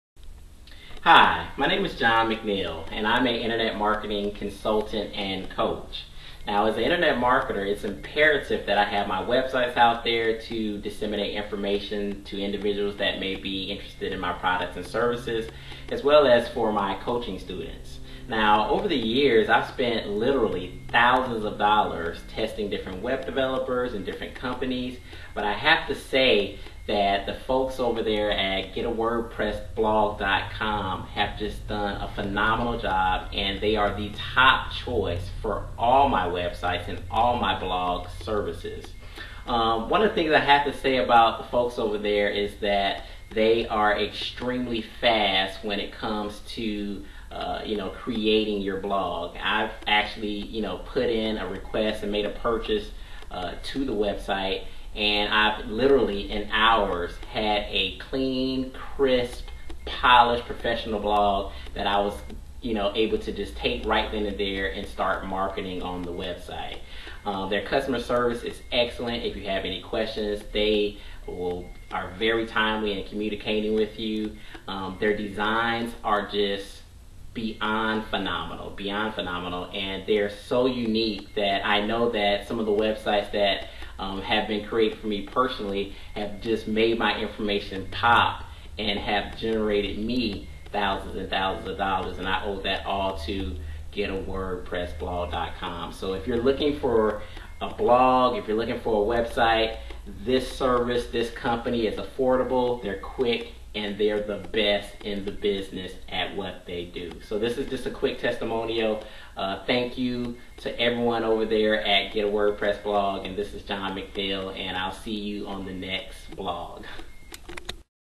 WordPress Blog Design: Jon McNeil Gives Advice
Jon McNeil is the owner of JonMcNeil.com and StreetSmartProfitsOffer.com.  Both websites were development by GetAWordPressBlog.com